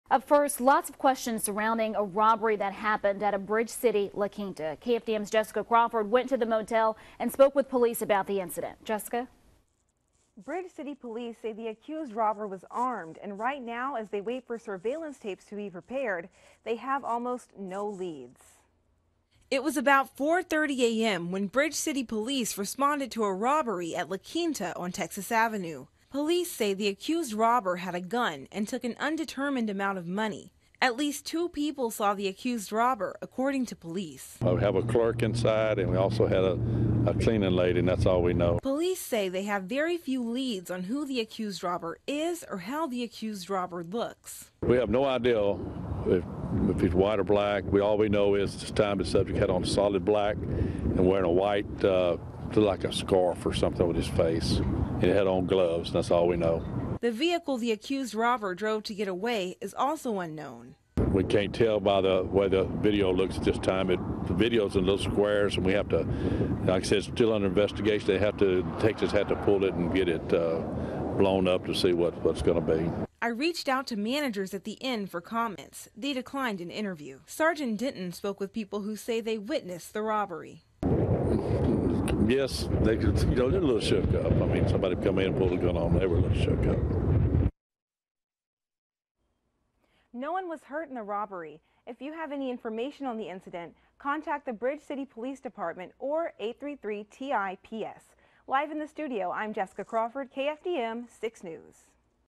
La Quinta in Bridge City robbed at gunpoint
BRIDGE CITY- by Jessica Crawford
Around 4:30 a.m., Bridge City Police say they responded to a robbery at La Quinta on Texas Avenue.
Police say witnesses tell them the robber had a gun, and took an undetermined amount of money.
According to police, a clerk and a cleaning lady witnessed the robbery.
Sgt. Ronnie Denton says he has very few leads on who robbed the motel.
"We have no idea if he's white or black," he says. "All we know is the subject had on solid black and wore a white.. it looked like a scarf.. on his face. He had on gloves. That's all we know."
Bridge City police are still waiting to view surveillance tapes to get a better look at the accused robber.
Sgt. Denton spoke with the people who say they witnessed the robbery.
"They're a little shook up," he says. "For somebody to come in and pull a gun on them.. they were a little shook up."
If you have any information on the robbery, contact the Bridge City Police Department or 833-TIPS.